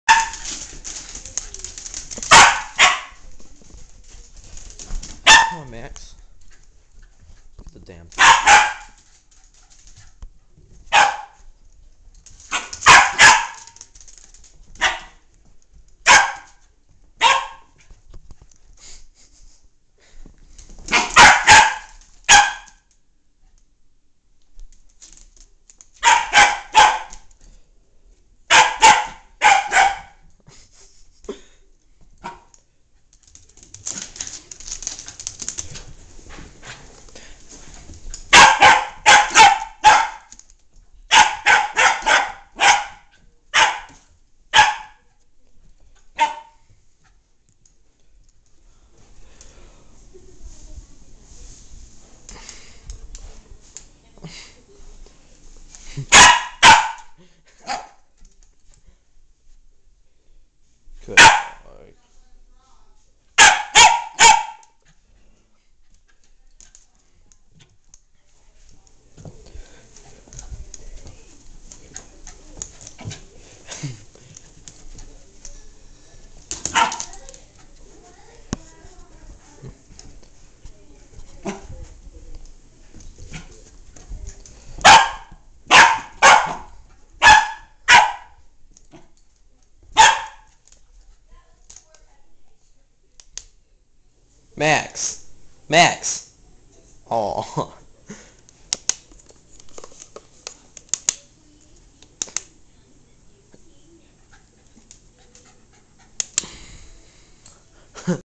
Puppy chases light
my welsh terrier puppy chases and attacks a light.